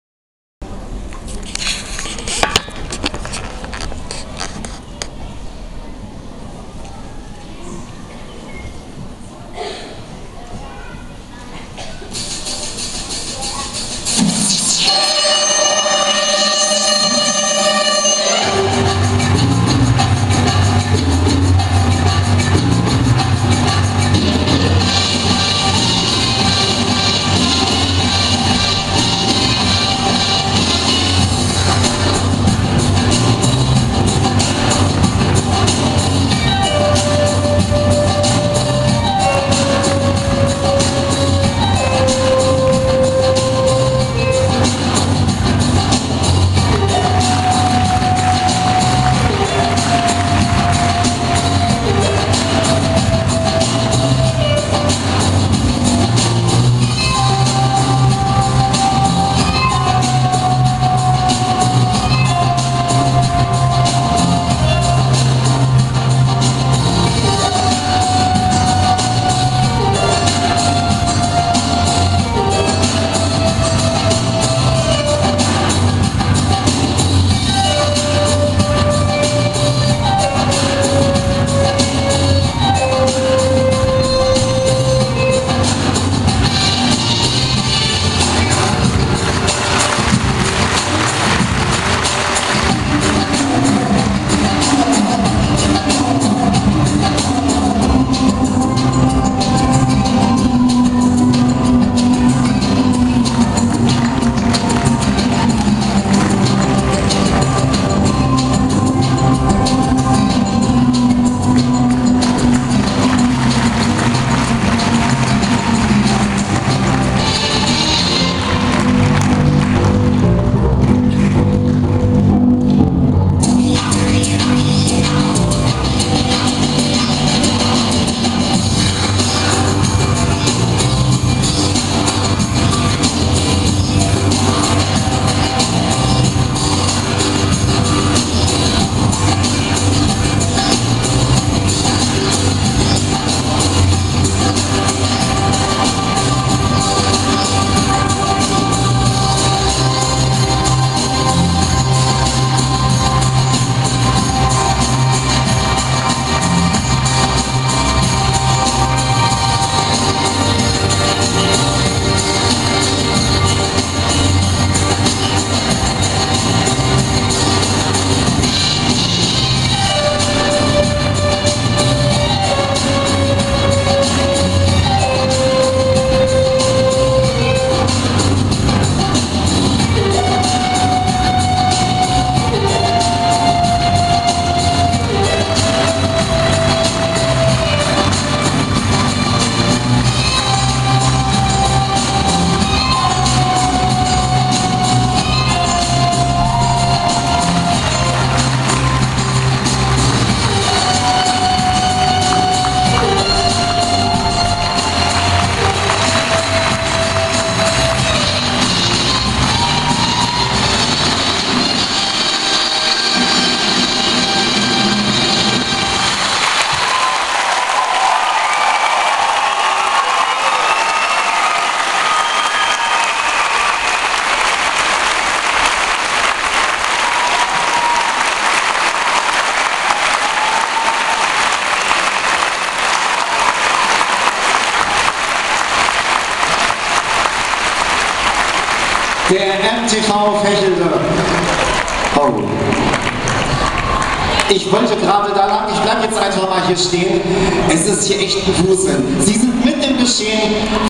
MTV Vechelde bei Magic Moments 2011 in Braunschweig
Turnen trifft Tanz trifft Akrobatik.
Unter dem Motto "Hollywood" haben sich verschiedene Vereine und Abteilungen bei den Magic Moments 2011 in der ausverkauften Braunschweiger Stadthalle präsentiert.